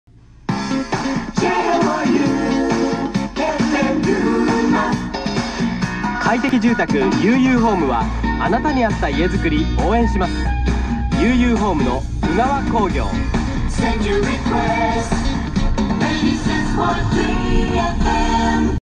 FMぐんま ジングル内CM 鵜川興業
平日朝8時45分ほどに流れます。